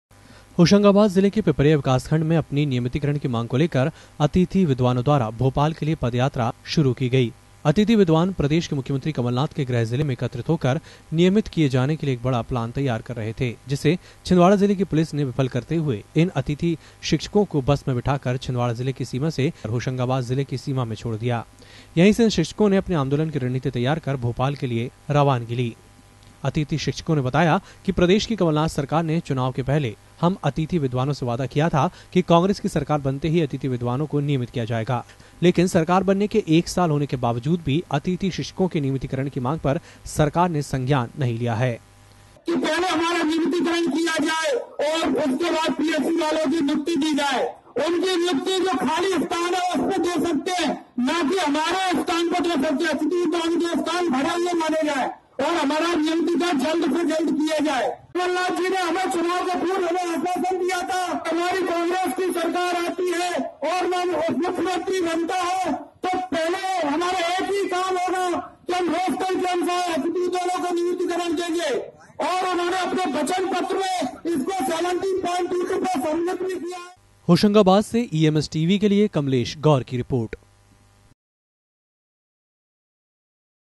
athiti sikshak 4 12 19
नियमितीकरण की मांग को लेकर अतिथि विद्वान भोपाल के लिए हुए रवाना